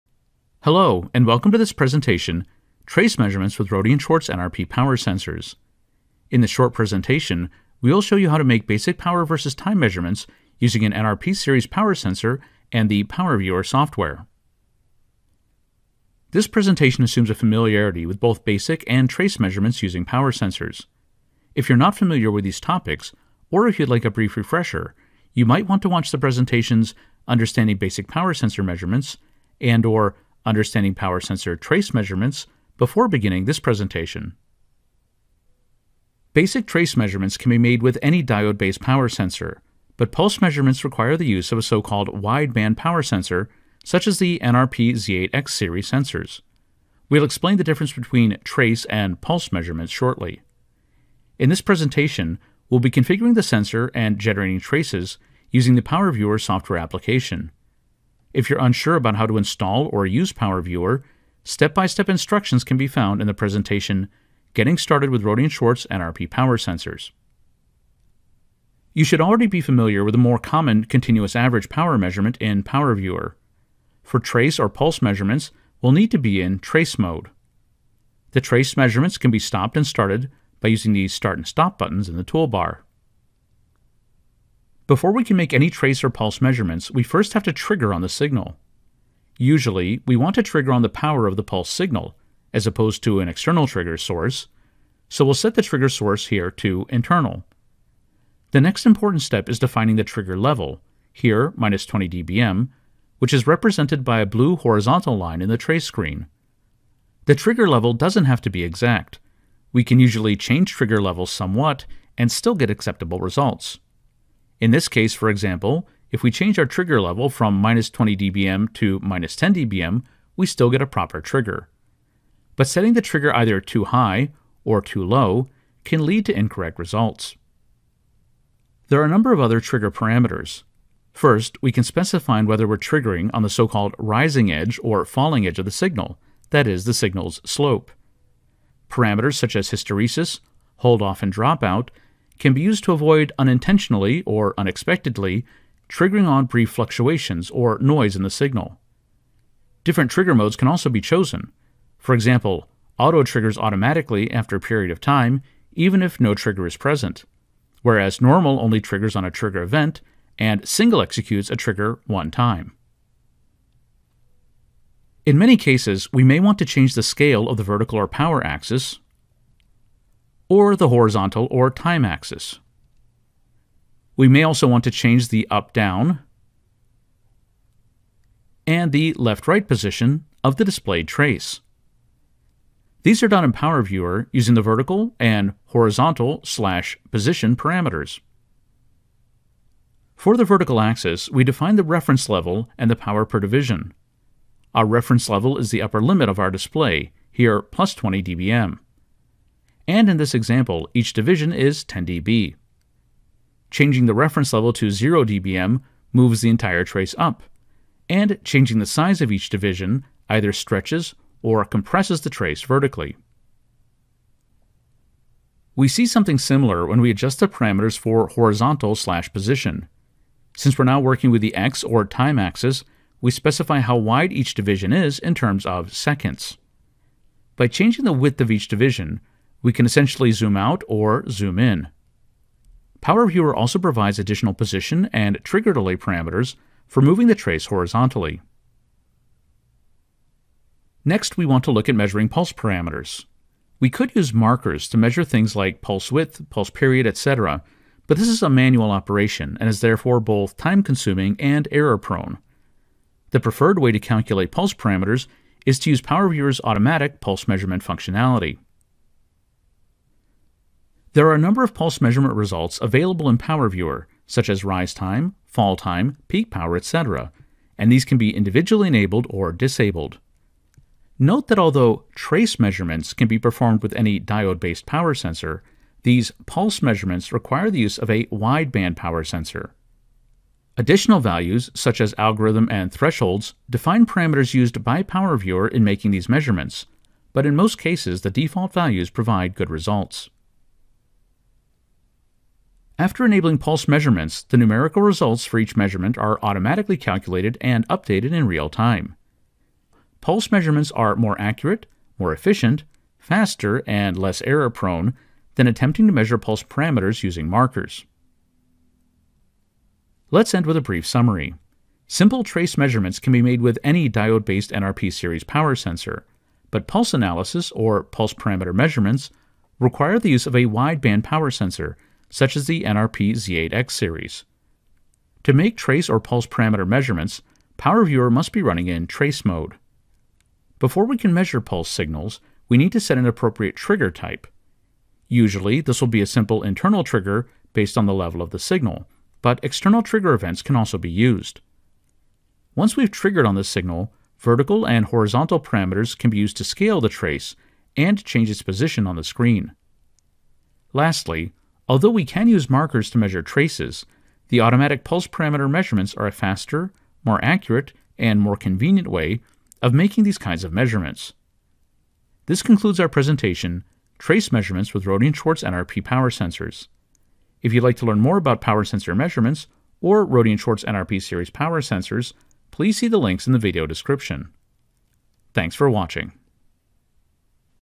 Trace Measurements with NRP Power Sensors
This video explains how to use Rohde and Schwarz NRP series power sensors to make trace and pulse measurements

Timeline:
00:00 Introduction
00:15 Suggested viewing
00:35 Hardware and software requirements
01:08 Power Viewer Trace Mode
01:23 Triggering
02:08 Additional triggering parameters
02:44 Vertical and horizontal position
03:09 Vertical
03:34 Horizontal / Position
04:00 Markers
04:20 Selecting pulse measurements
04:52 Pulse measurement results
05:09 Summary